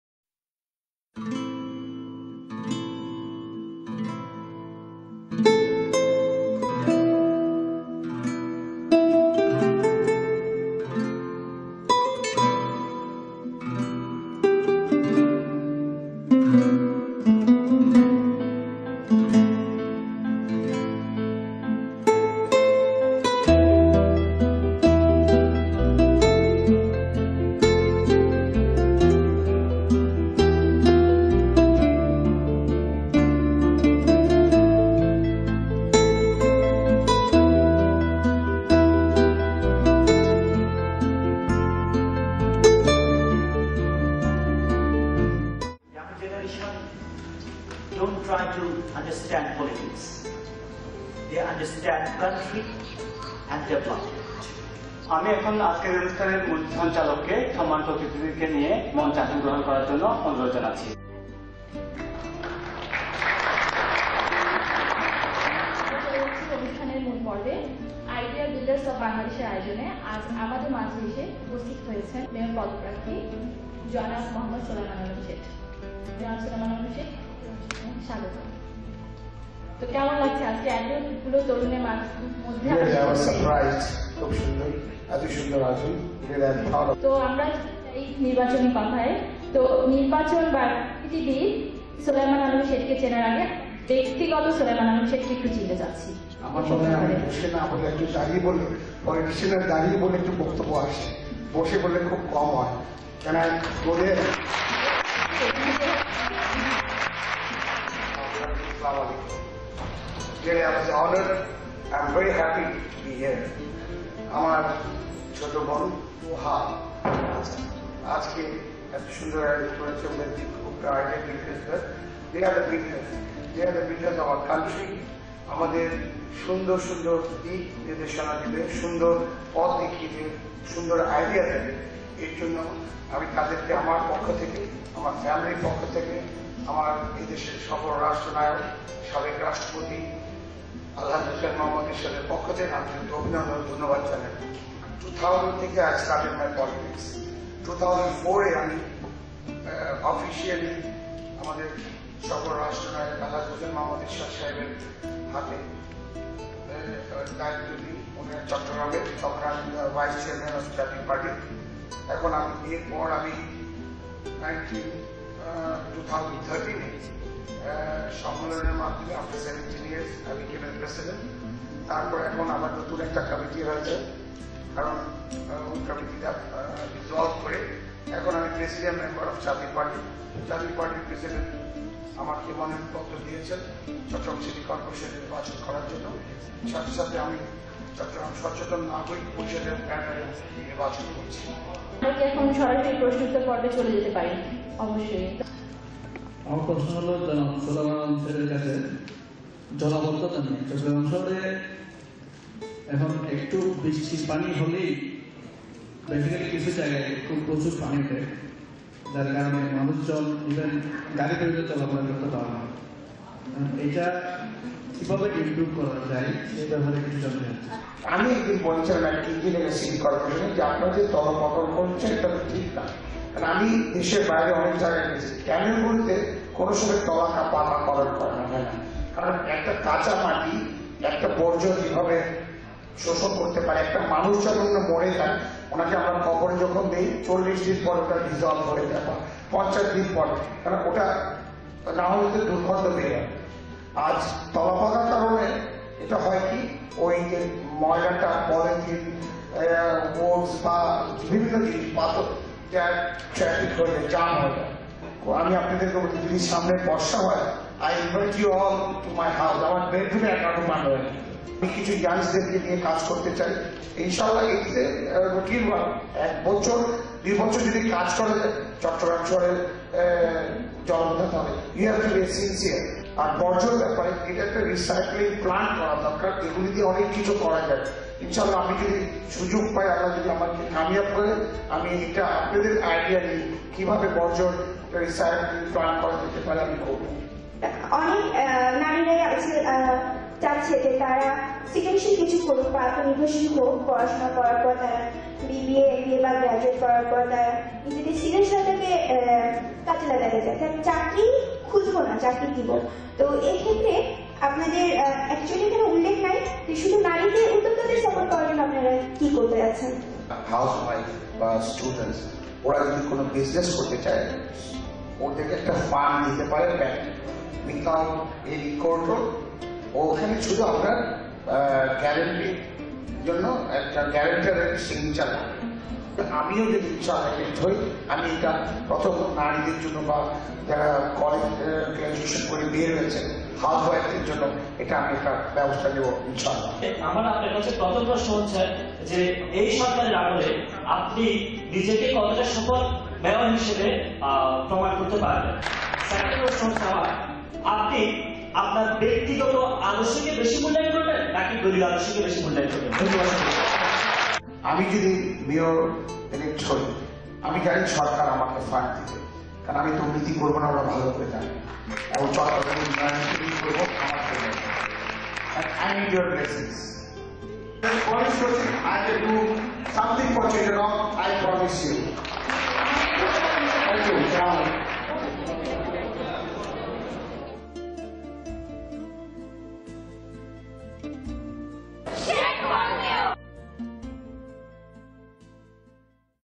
Face to Face Solaiman Alam Seth
Face to Face Solaiman Alam Seth at Radisson Blu Chittagong Bay View in Chittagong.